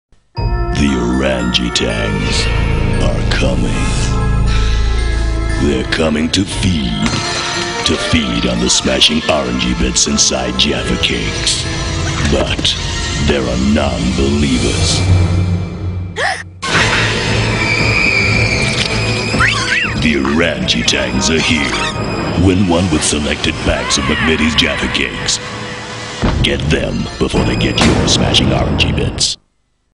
Jaffa Cakes - Orangey Tangs (1997, UK)
An advert featuring the short-lived Orangey Tangs, the little orange fuzzballs who craved the orangey filling inside the Jaffa Cakes.